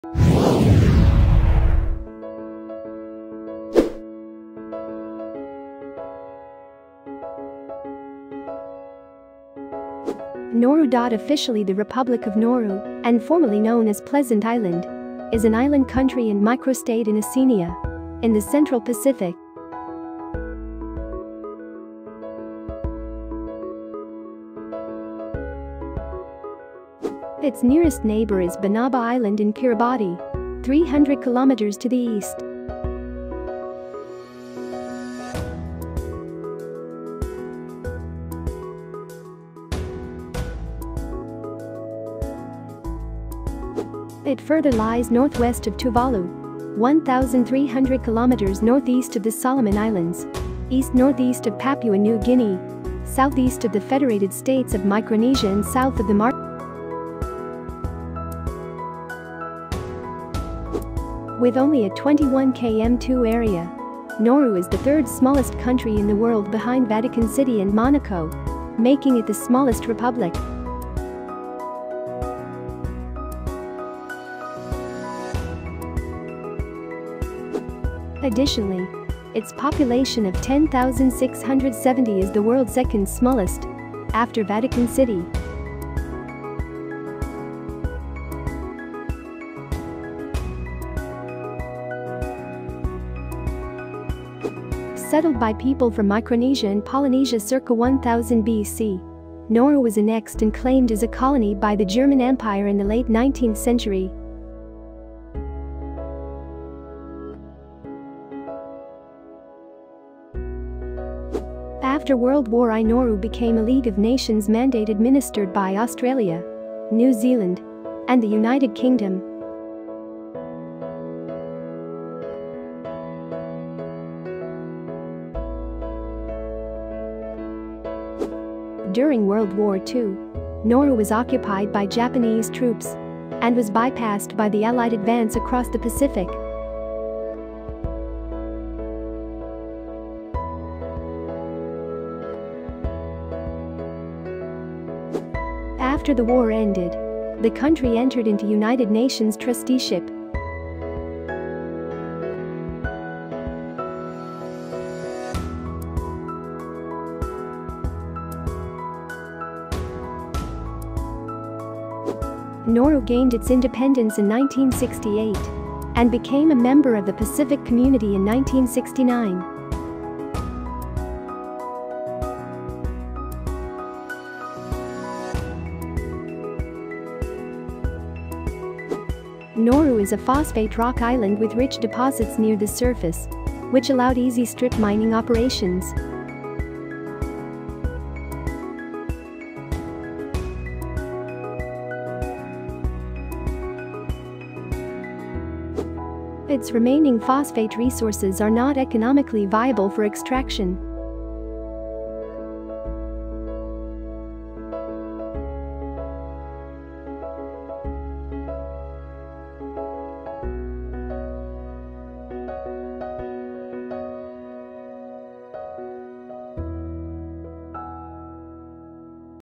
The history of Nauru. Nauru documentary. World Of Knowledge
Nauru , officially the Republic of Nauru and formerly known as Pleasant Island, is an island country and microstate in Oceania, in the Central Pacific.

Its nearest neighbour is Banaba Island in Kiribati, 300 km to the east.

It further lies northwest of Tuvalu, 1,300 km northeast of the Solomon Islands, east-northeast of Papua New Guinea, southeast of the Federated States of Micronesia and south of the Marshall Islands.

With only a 21 km2 area, Nauru is the third-smallest country in the world behind Vatican City and Monaco, making it the smallest republic.

Additionally, its population of 10,670 is the world's second smallest, after Vatican City.

Settled by people from Micronesia and Polynesia circa 1000 BC, Nauru was annexed and claimed as a colony by the German Empire in the late 19th century.

After World War I, Nauru became a League of Nations mandate administered by Australia, New Zealand, and the United Kingdom.

During World War II, Nauru was occupied by Japanese troops, and was bypassed by the Allied advance across the Pacific.

After the war ended, the country entered into United Nations trusteeship.

Nauru gained its independence in 1968, and became a member of the Pacific Community in 1969.

Nauru is a phosphate-rock island with rich deposits near the surface, which allowed easy strip mining operations.

Its remaining phosphate resources are not economically viable for extraction.